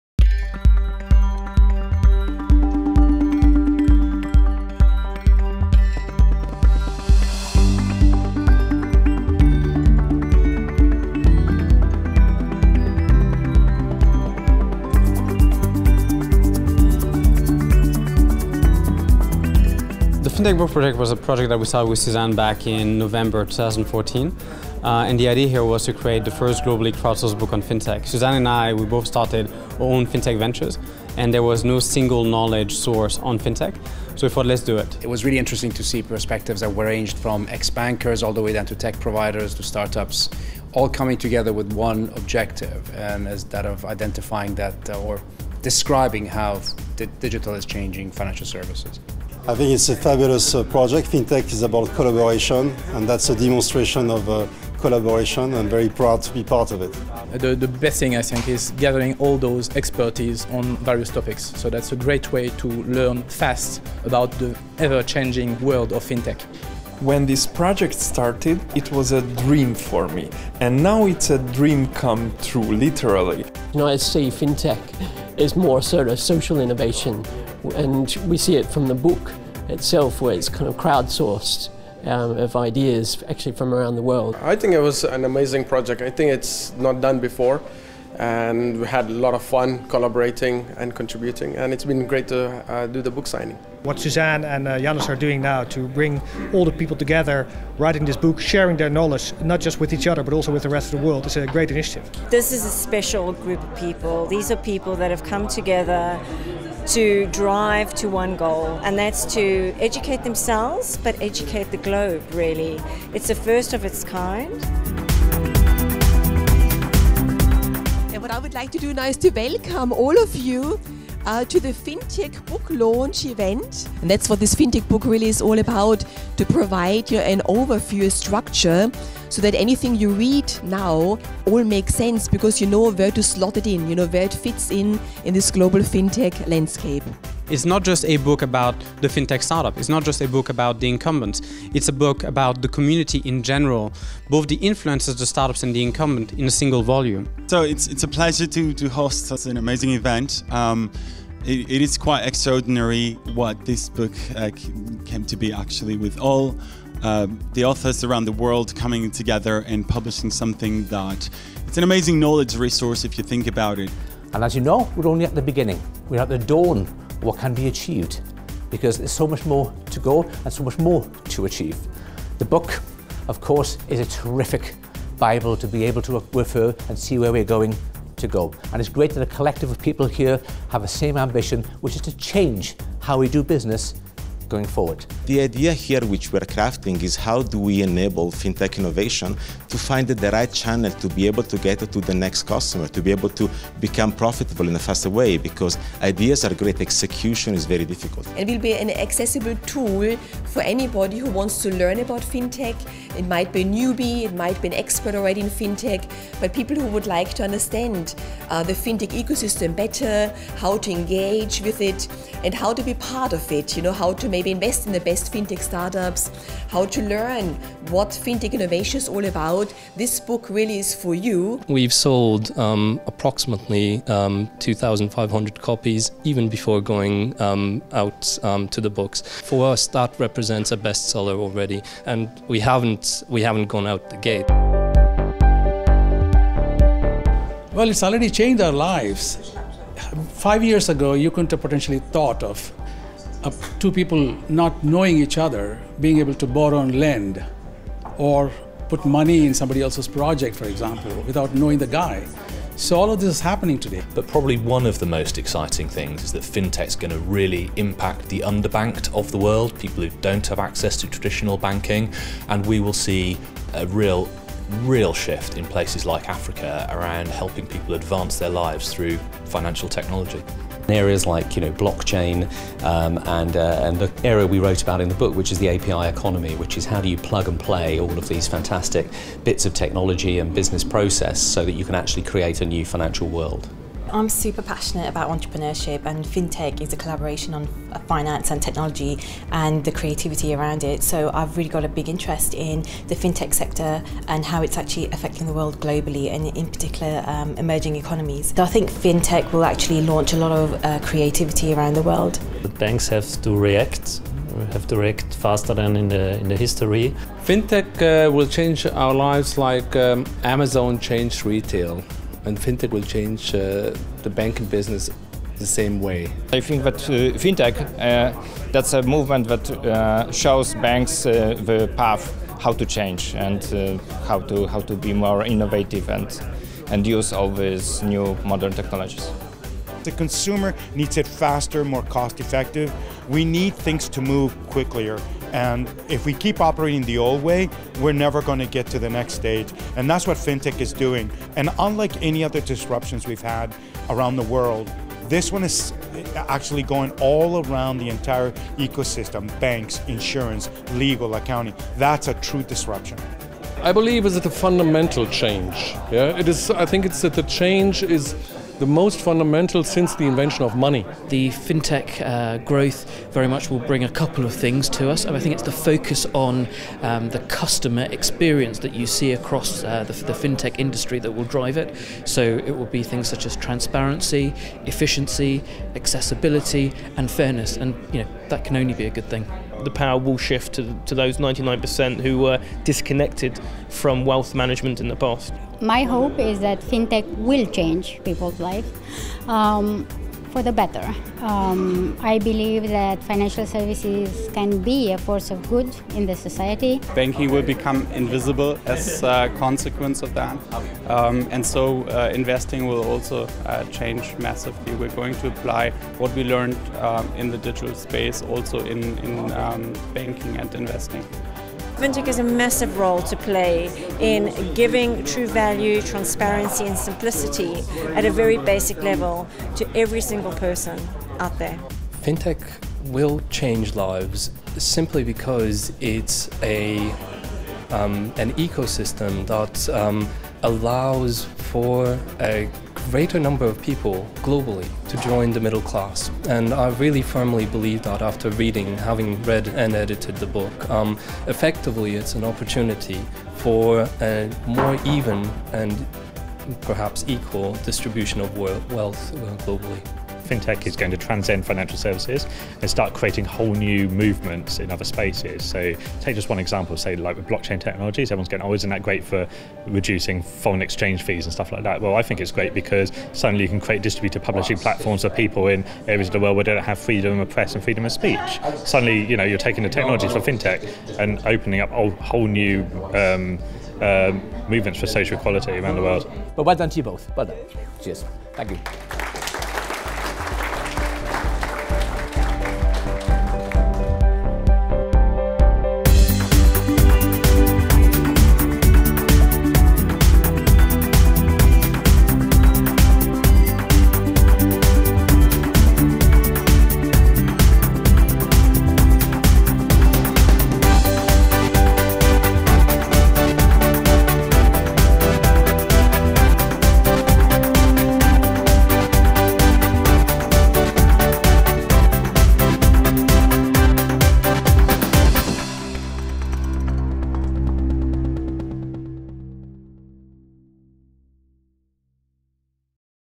The FINTECH Book - Global Book Launch in London
Join us in the global Book Launch event of the Bestseller "The FINTECH Book".